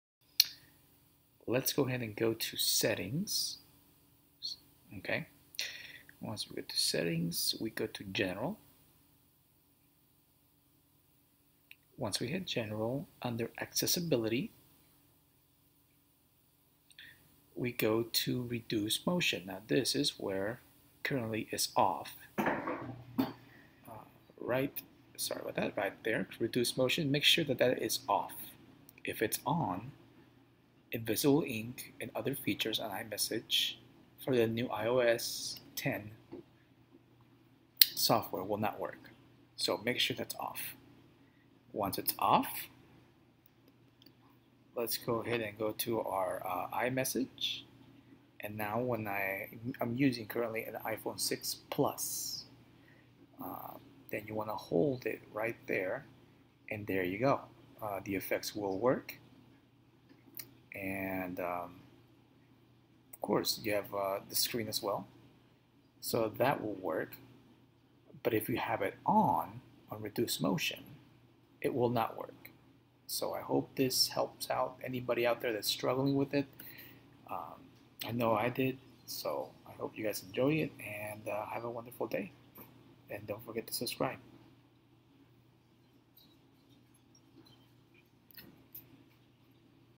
How To Fix or Enable Invisible Ink on IOS 10 IMessage!!
Today, I am posting a video on how to enable or fix this problem that has cause many users not to use invisible ink on Imessage, I hope this tutorial helps.